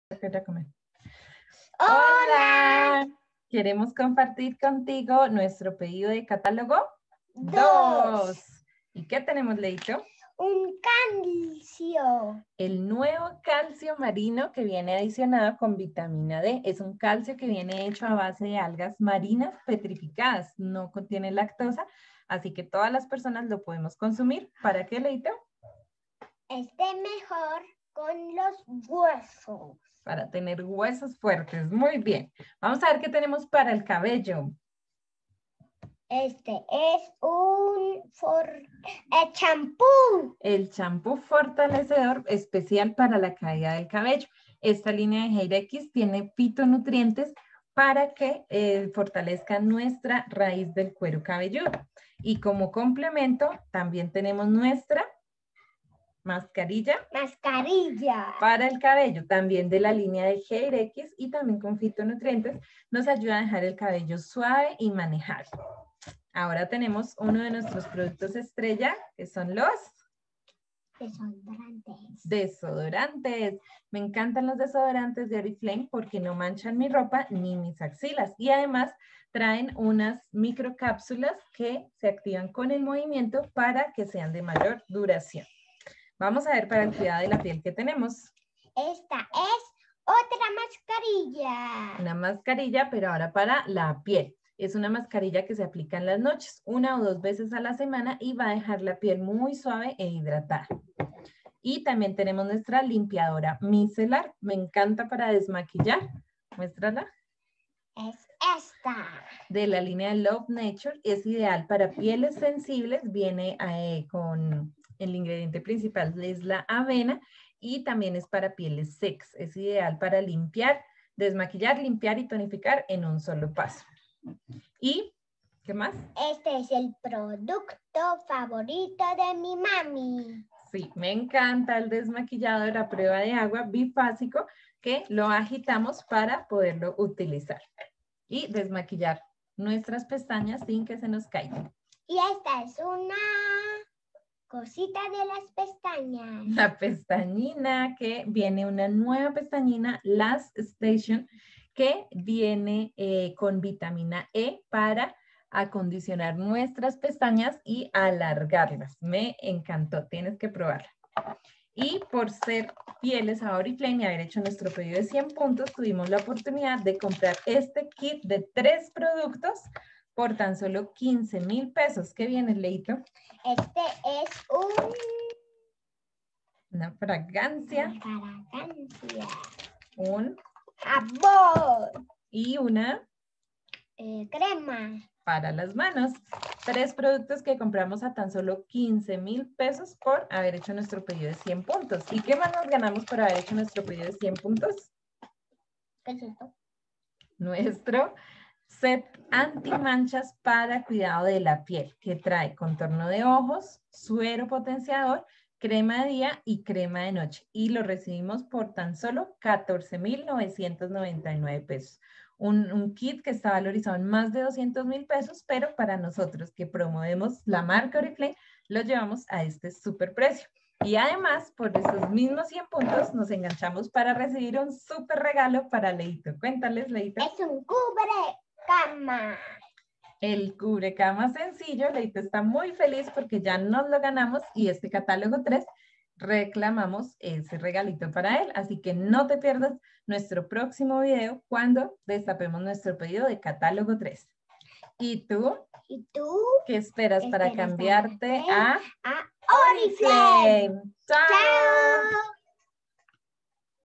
Unboxing C2 - Deissy y Leito
Conoce los productos que recibimos en nuestro pedido de C2 y los premios por ser Embajadores de Oriflame!!!